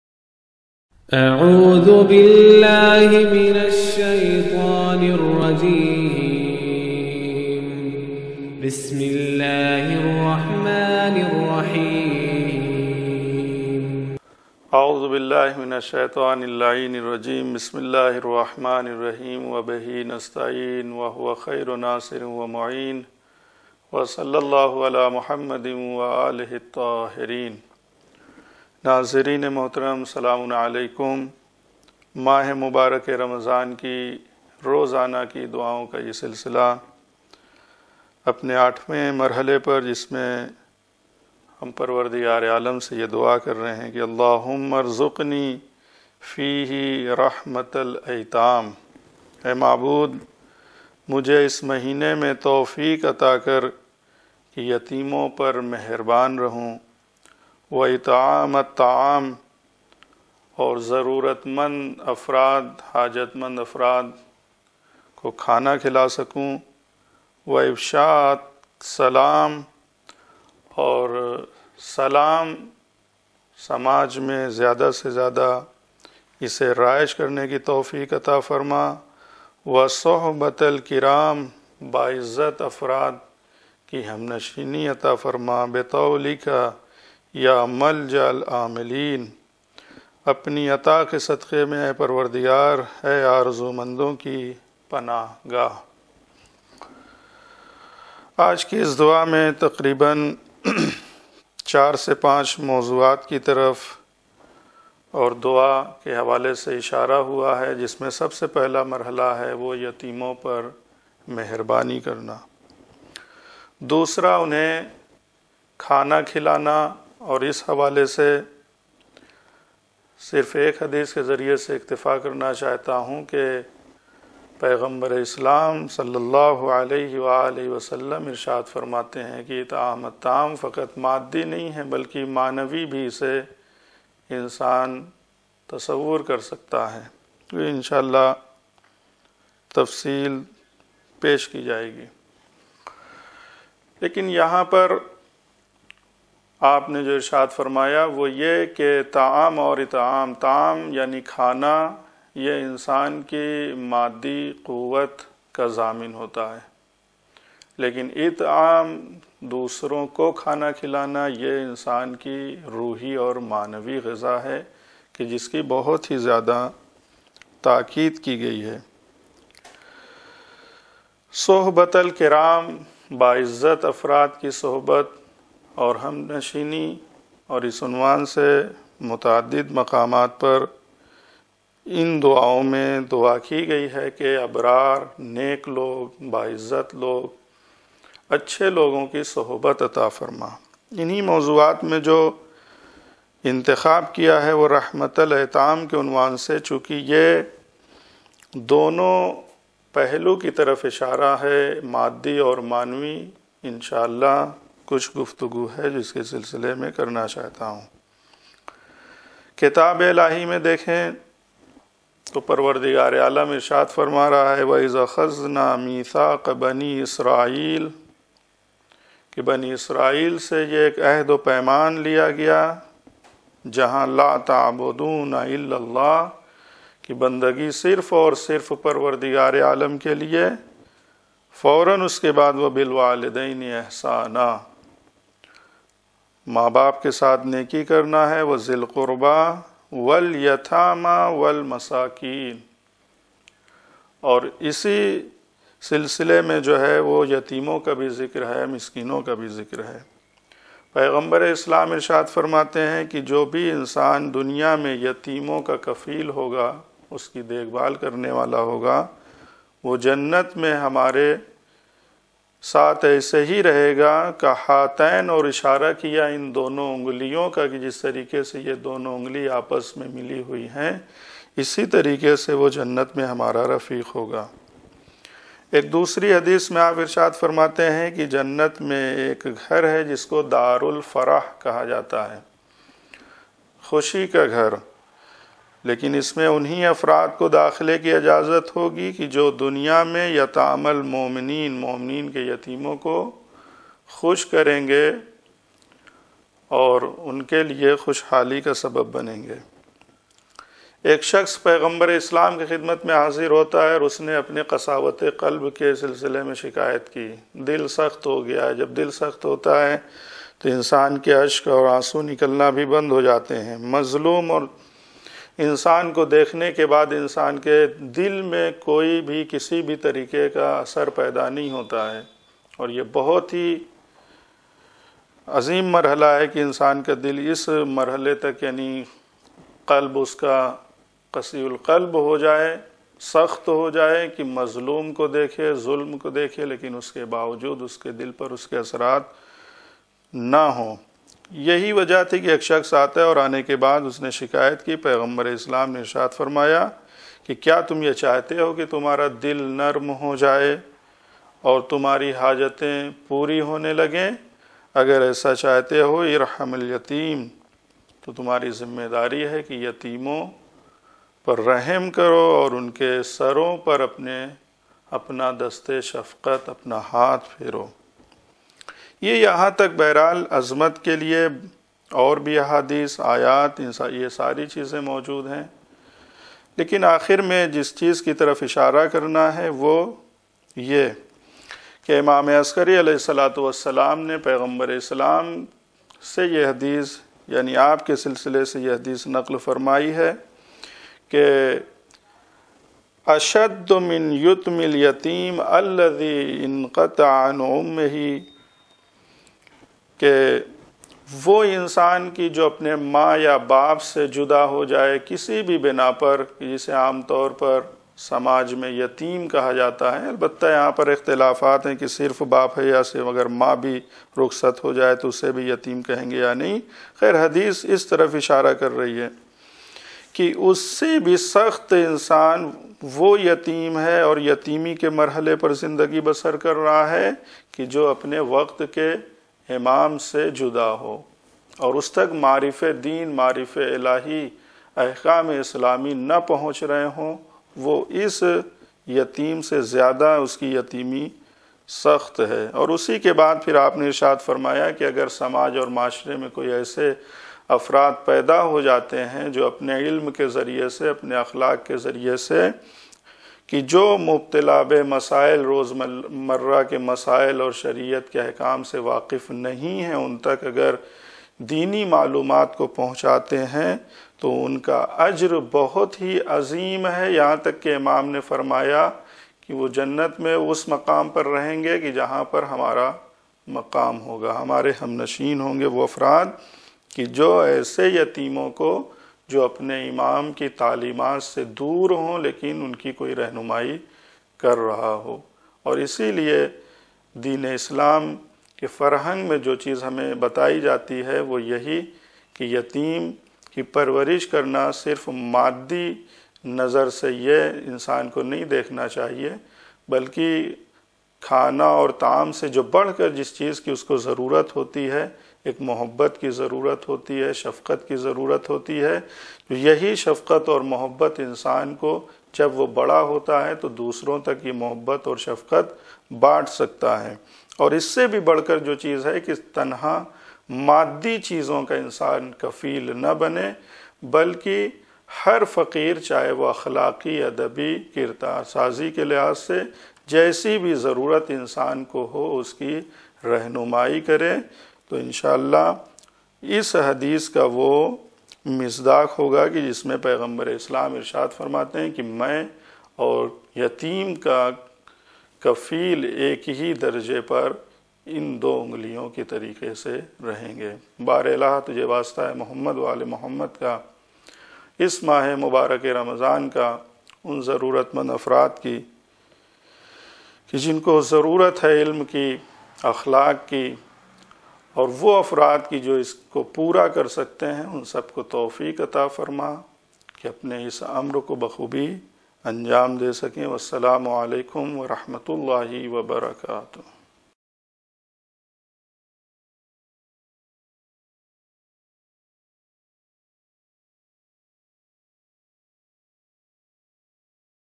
Day 08, Mahe Ramadhan Daily Dua, (Mahe Ramzan ki Rozana ki Dua) | Maulana Alamdar Hasnain Rizvi
Yatimo Ka Ikraam Mahe Ramzan ki Rozana ki Dua ki Roshni me  (Day 08)
Maulana Alamdar Hasnain Rizvi

Brief Explanation of Daily Dua of Mahe Ramadhan
یتیم کا اکرام  ماہ رمضان کی روزانہ کی دعاؤں کی روشنی میں(ماہ  رمضان کے آٹھویں  دن کی دعا)